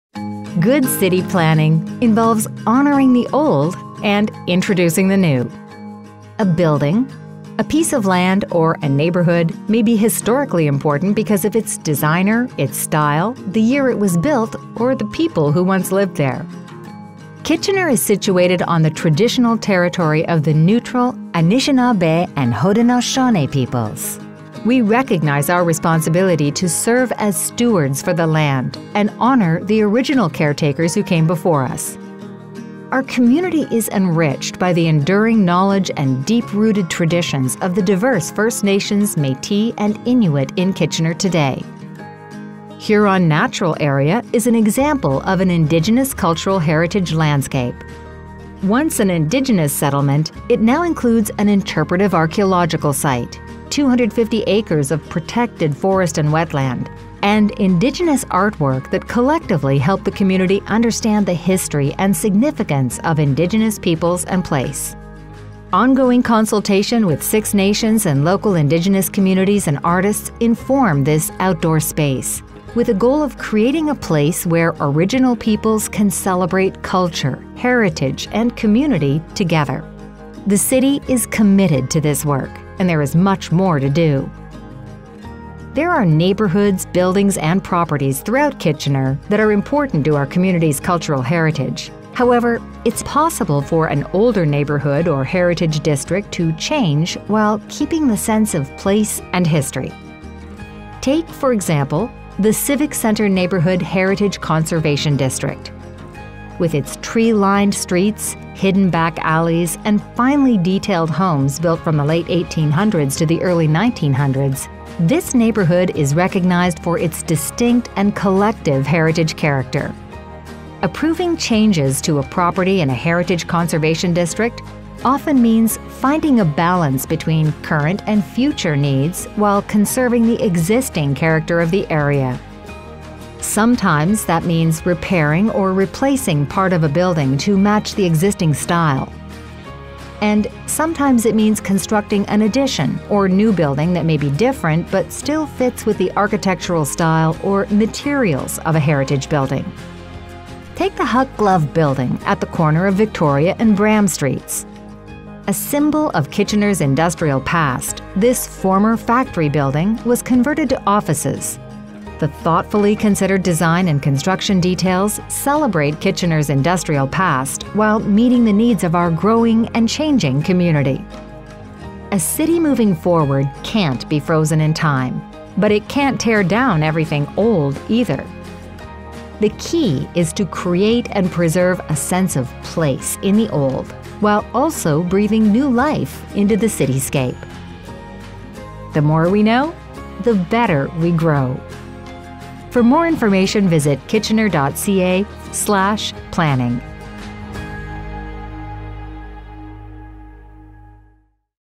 Cultural heritage
Good city planning involves honouring the old while welcoming the new. A building, piece of land or neighbourhood can be historically significant for a variety of reasons. It’s possible for these places to change somewhat while retaining its sense of place and history. The City of Kitchener works to strike this balance – conserving our heritage while meeting the needs of a growing community.